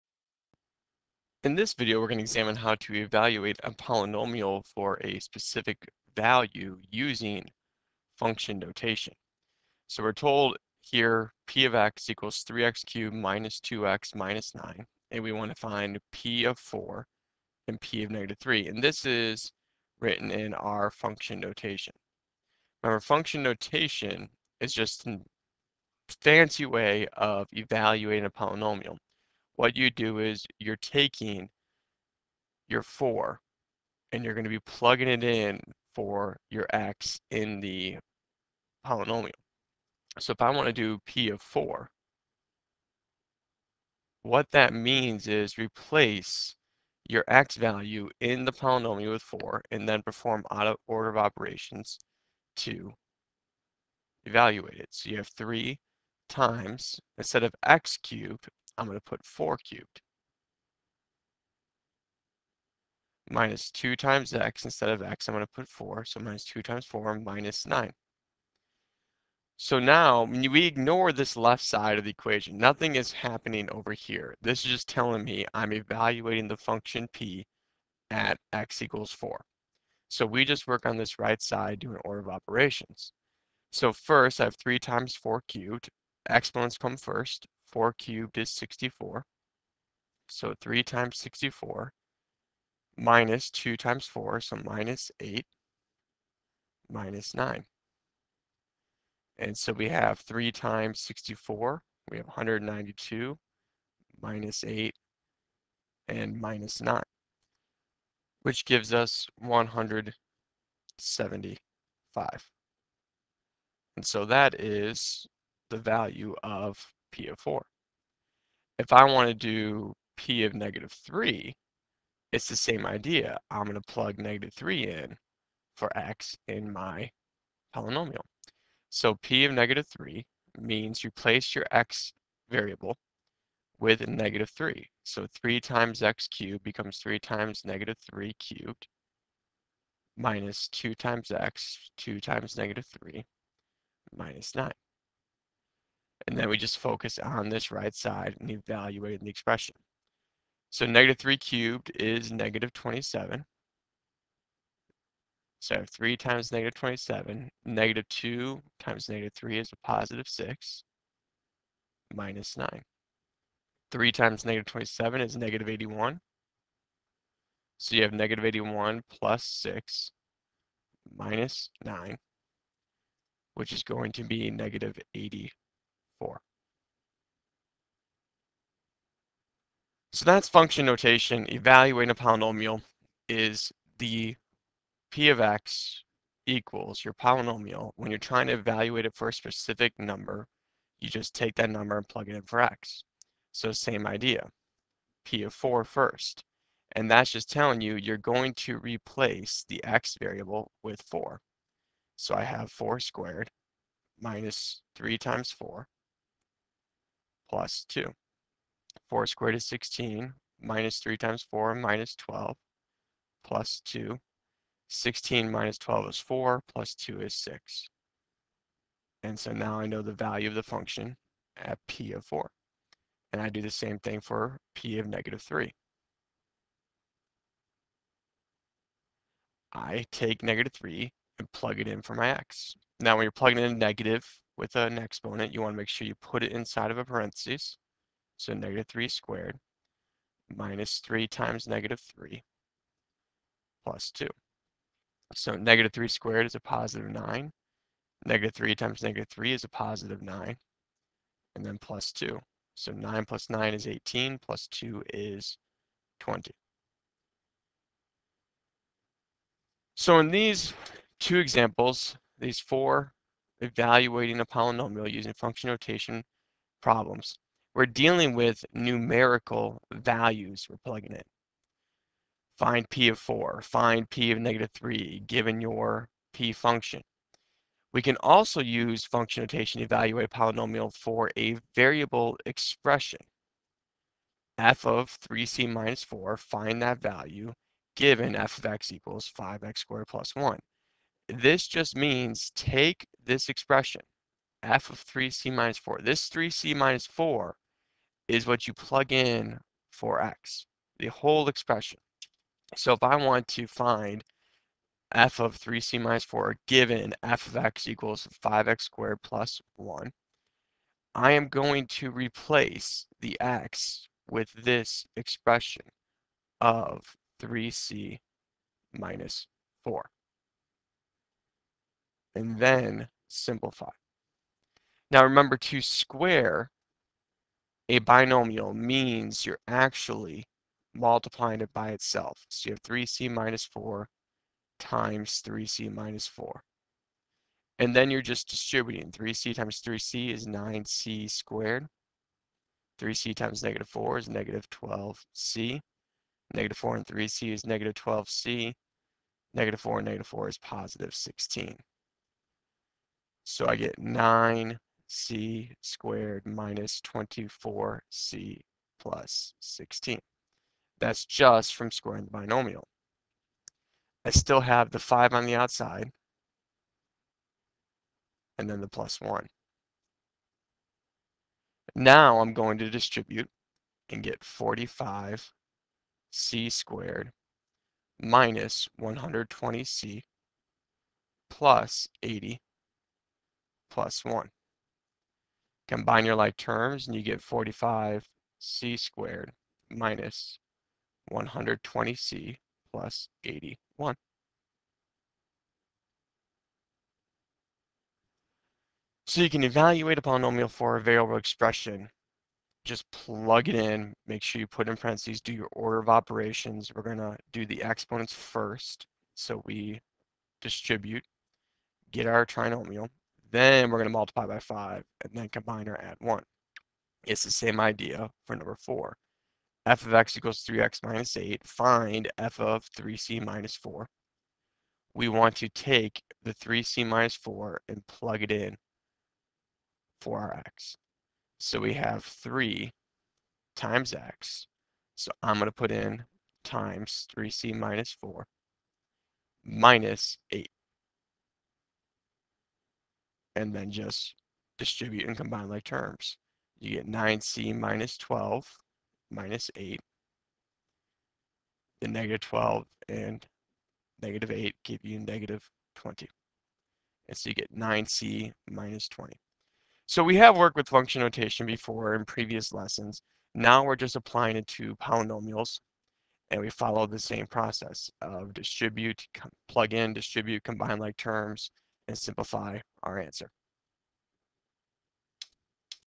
Lesson 6.3 - Evaluating a Polynomial Using Function Notation
The following video examines how to evalaute a polynomial function for a specific numerical value using function notation.  There are also two examples that examine how to evaluate a polynomial function using function notation for a variable expression.